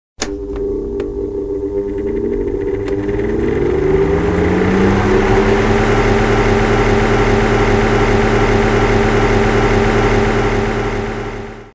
ventilateur
Essai du flou de mouvement et du clignotement avec le tag python